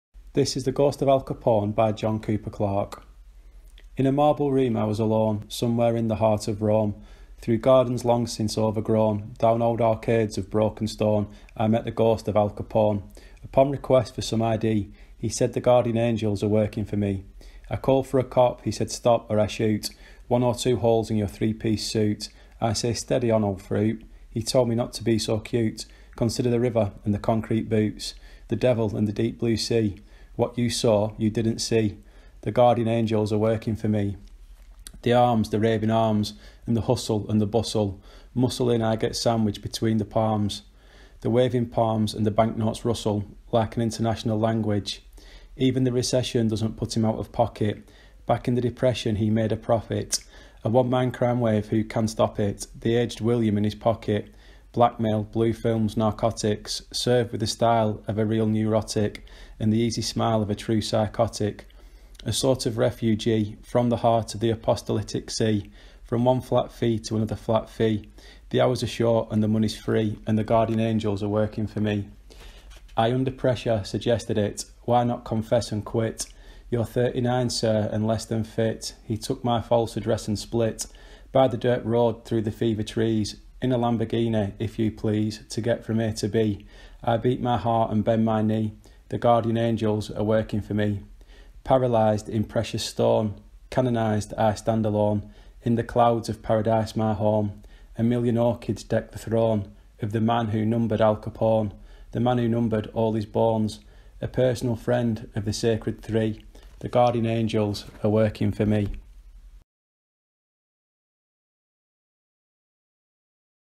The Ghost of Al Capone by John Cooper Clarke
Read by Mr Baiamonte